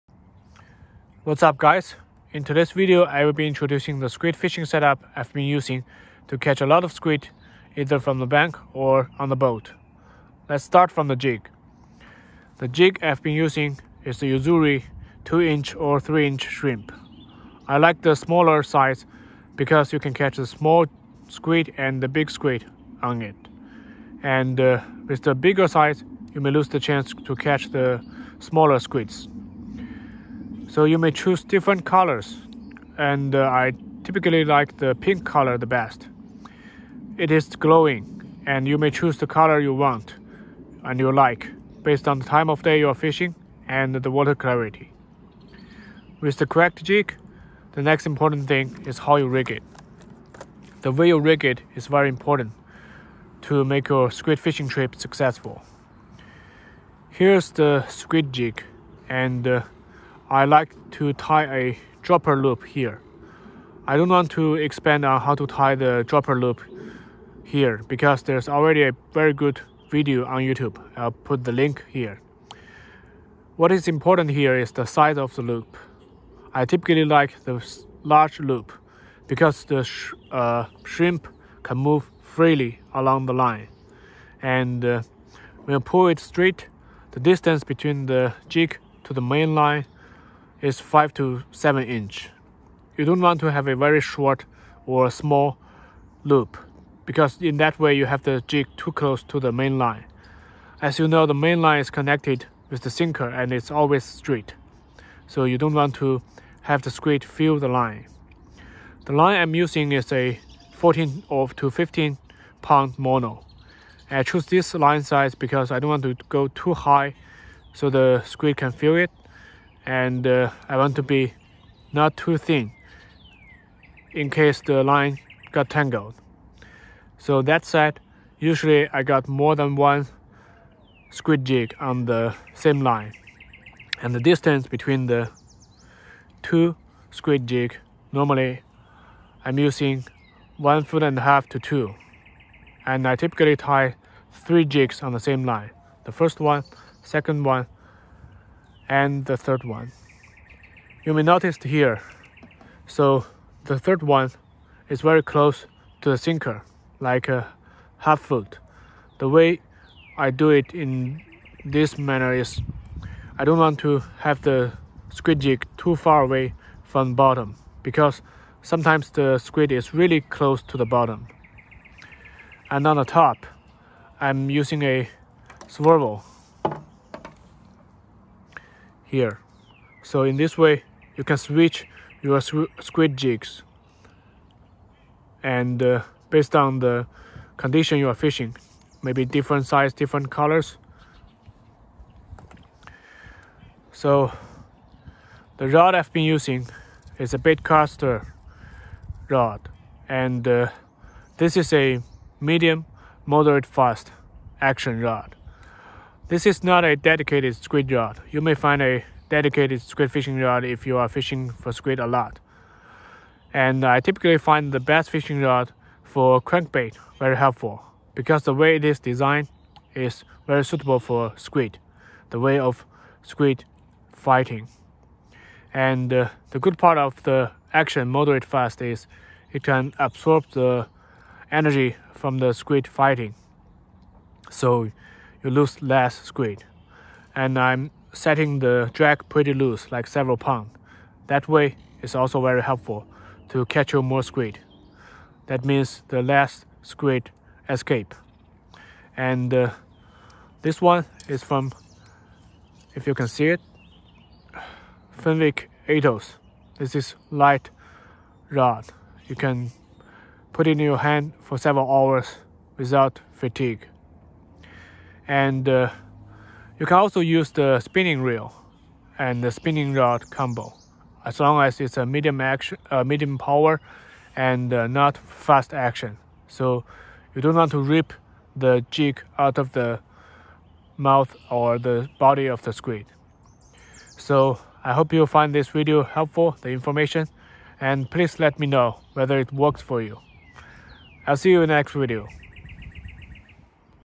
Rig/Setup that Catches MORE Squid
In this video I will share with you my squid fishing setup that can catch you more squid!